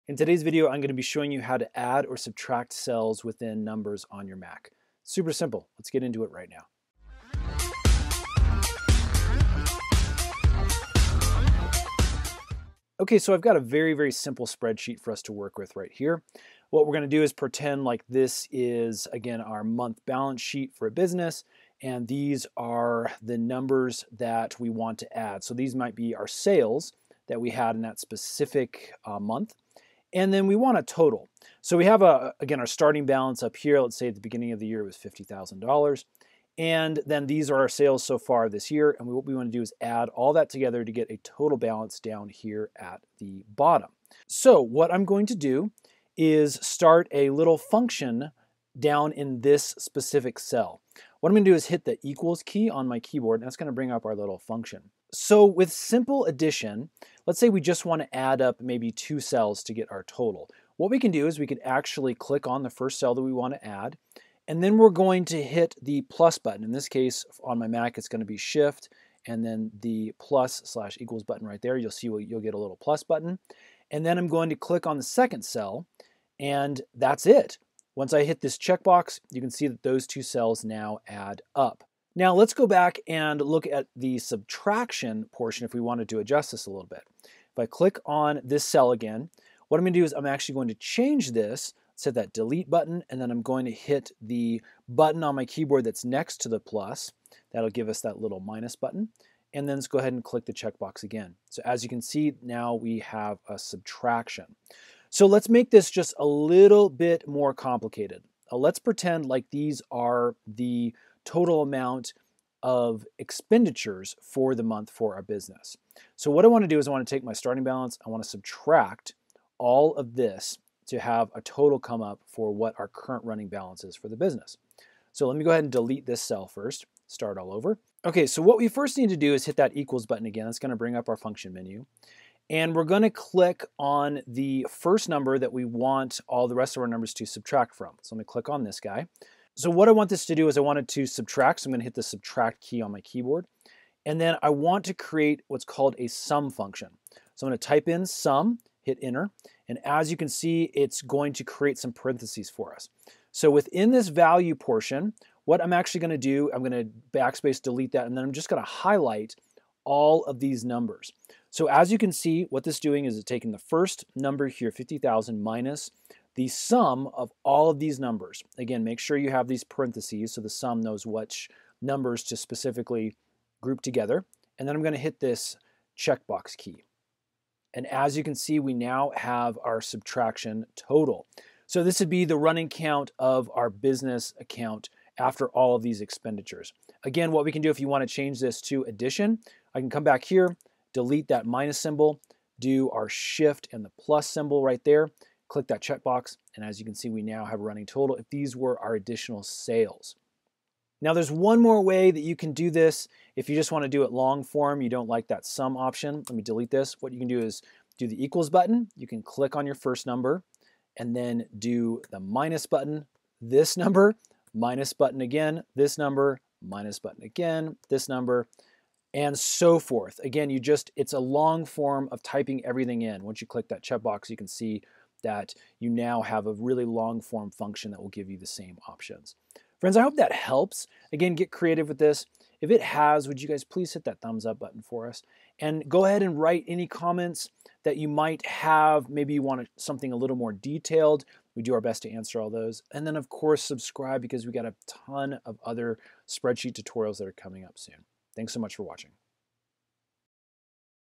How to Add or Subtract in Apple Numbers
Check out today's special tech deals:

In this video, we show you how to add or subtract in Apple numbers. This is a very easy process and can be done multiple ways using selected cells or groupings of cells. We walk you through the process to add and subtract step-by-step so you can perform the function in your own Apple Numbers spreadsheets.